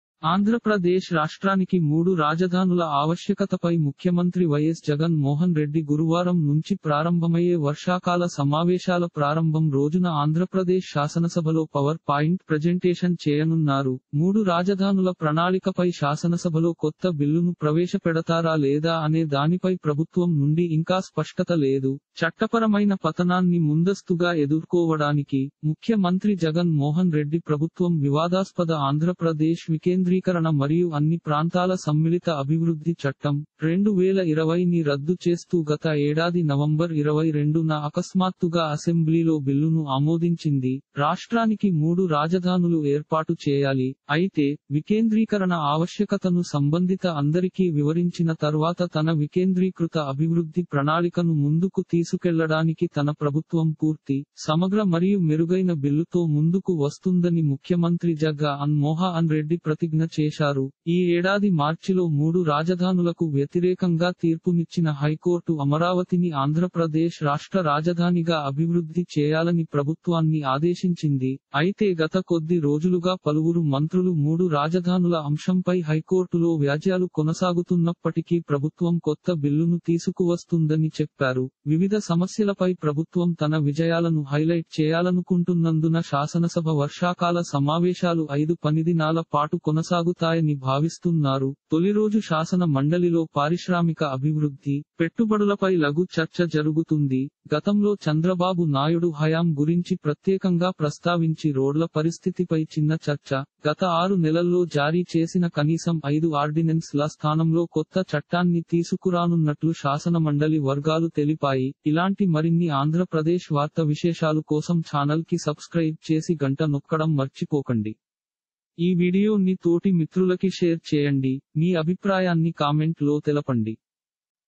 Ap Cm Jagan Mohan Reddy Key Decision Regarding Three Capitals For Ap Details - ఏపీ మూడు రాజ‌ధానుల‌
ఆంధ్ర‌ప్ర‌దేశ్ రాష్ట్రానికి మూడు రాజధానుల ఆవశ్యకతపై ముఖ్యమంత్రి వైఎస్‌ జగన్‌మోహన్‌రెడ్డి గురువారం నుంచి ప్రారంభమయ్యే వర్షాకాల సమావేశాల ప్రారంభం రోజున ఆంధ్రప్రదేశ్‌ శాసనసభలో పవర్‌పాయింట్‌ ప్రజెంటేషన్‌ చేయనున్నారు. మూడు రాజధానుల ప్రణాళికపై శాసనసభలో కొత్త బిల్లును ప్రవేశపెడతారా లేదా అనే దానిపై ప్రభుత్వం నుండి ఇంకా స్పష్టత లేదు.

చట్టపరమైన పతనాన్ని ముందస్తుగా ఎదుర్కోవడానికి, ముఖ్య‌మంత్రి జగన్ మోహన్ రెడ్డి ప్రభుత్వం వివాదాస్పద ఆంధ్ర‌ప్ర‌దేశ్ వికేంద్రీకరణ మరియు అన్ని ప్రాంతాల సమ్మిళిత అభివృద్ధి చట్టం, 2020ని రద్దు చేస్తూ గత ఏడాది నవంబర్ 22న అకస్మాత్తుగా అసెంబ్లీలో బిల్లును ఆమోదించింది. రాష్ట్రానికి మూడు రాజధానులు ఏర్పాటు చేయాలి.

అయితే, వికేంద్రీకరణ ఆవశ్యకతను సంబంధిత అందరికీ వివరించిన తర్వాత తన వికేంద్రీకృత అభివృద్ధి ప్రణాళికను ముందుకు తీసుకెళ్లడానికి తన ప్రభుత్వం పూర్తి, సమగ్ర మరియు మెరుగైన బిల్లుతో ముందుకు వస్తుందని ముఖ్యమంత్రి జ‌గ‌న్ మోహ‌న్ రెడ్డి ప్రతిజ్ఞ చేశారు. ఈ ఏడాది మార్చిలో మూడు రాజధానులకు వ్యతిరేకంగా తీర్పునిచ్చిన హైకోర్టు అమరావతిని ఆంధ్ర‌ప్ర‌దేశ్ రాష్ట్ర రాజధానిగా అభివృద్ధి చేయాలని ప్రభుత్వాన్ని ఆదేశించింది.

అయితే గత కొద్ది రోజులుగా పలువురు మంత్రులు మూడు రాజధానుల అంశంపై హైకోర్టులో వ్యాజ్యాలు కొనసాగుతున్నప్పటికీ ప్రభుత్వం కొత్త బిల్లును తీసుకువస్తుందని చెప్పారు. వివిధ సమస్యలపై ప్రభుత్వం తన విజయాలను హైలైట్ చేయాలనుకుంటున్నందున శాసనసభ వర్షాకాల సమావేశాలు ఐదు పనిదినాల పాటు కొనసాగుతాయని భావిస్తున్నారు. తొలిరోజు శాసన మండలిలో పారిశ్రామిక అభివృద్ధి, పెట్టుబడులపై లఘు చర్చ జరుగుతుంది. గతంలో చంద్రబాబు నాయుడు హయాం గురించి ప్రత్యేకంగా ప్రస్తావించి రోడ్ల పరిస్థితిపై చిన్న చర్చ. గత ఆరు నెలల్లో జారీ చేసిన కనీసం ఐదు ఆర్డినెన్స్‌ల స్థానంలో కొత్త చట్టాన్ని తీసుకురానున్నట్లు శాసనమండలి వర్గాలు తెలిపాయి. - Ap Cm Jagan Mohan Reddy Key Decision Regarding Three Capitals For Ap Details $telugu-title:ఏపీ మూడు రాజ‌ధానుల‌పై సీఎం జ‌గ‌న్ మ‌రో కీల‌క నిర్ణ‌యం$ *pid:2149995* - Andhra Pradesh AndhraPradesh Andhra Amaravathi Telugu TeluguStop | Andhrapradesh